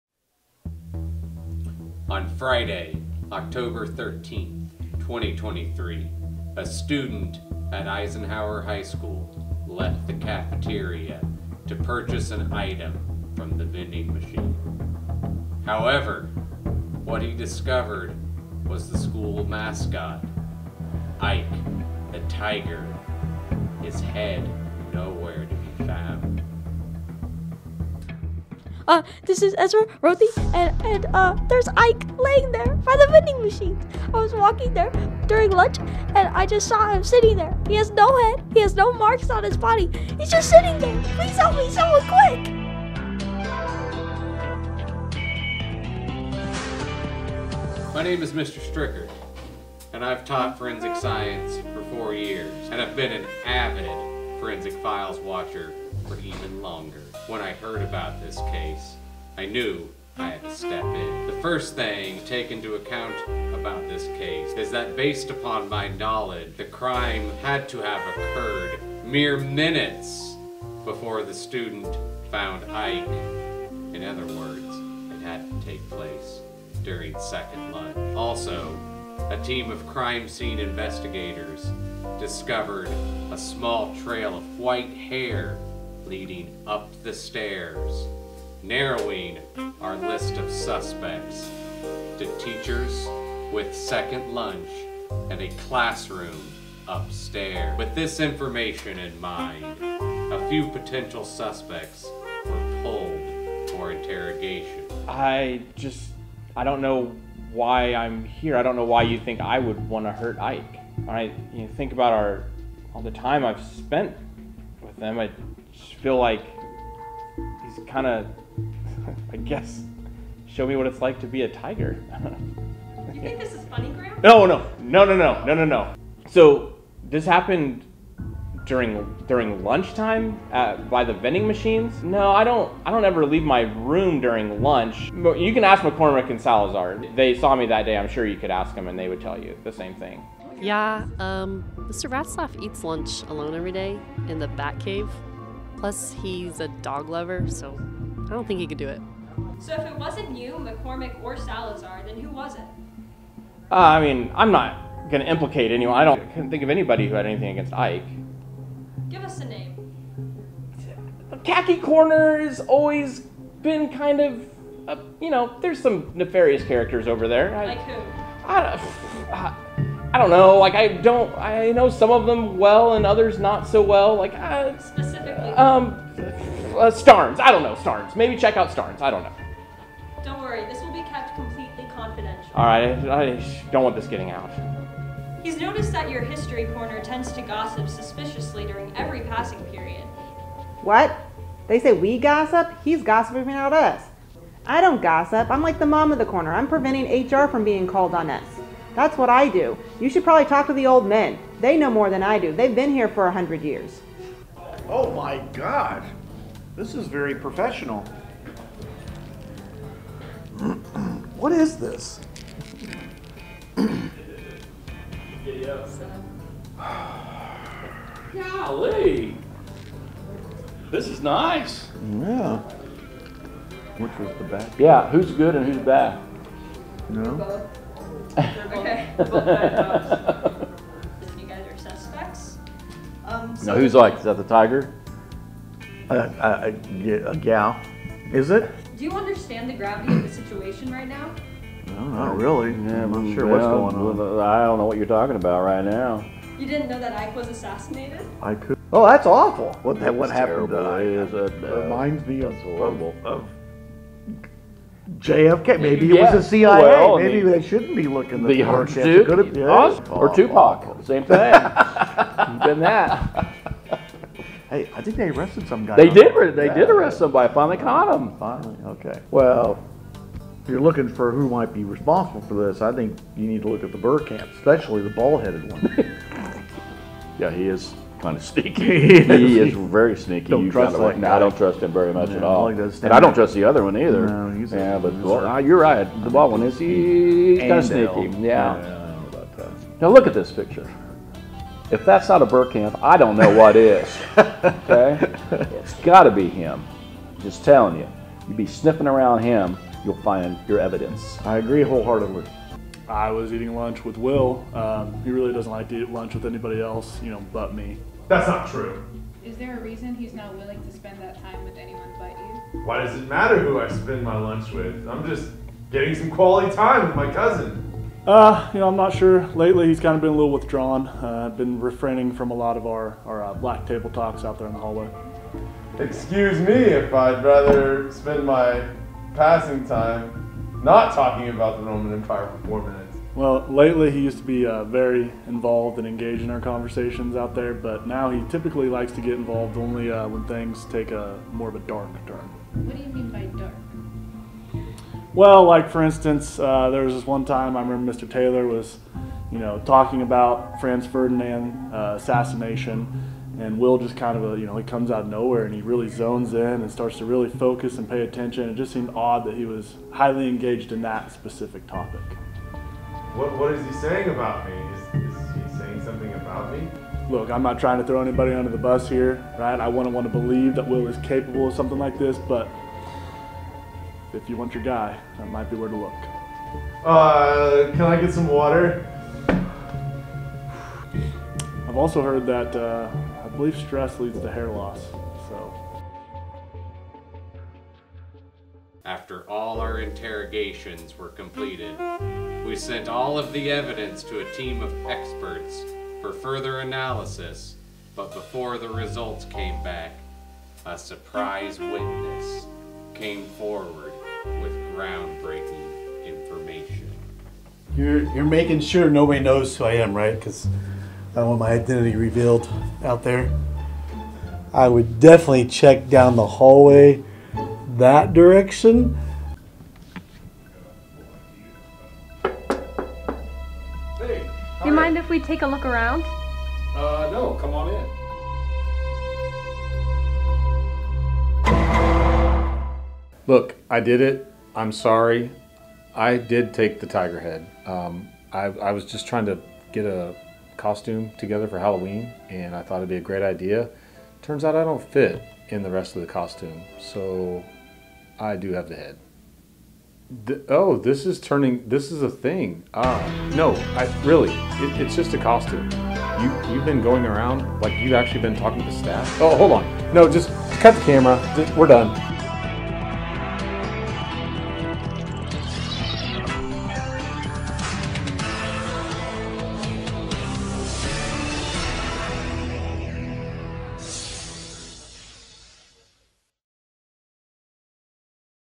EHS Forensic Files Halloween Mystery 2023
Eisenhower High School Ike Media presents the EHS Forensic Files Halloween Mystery special.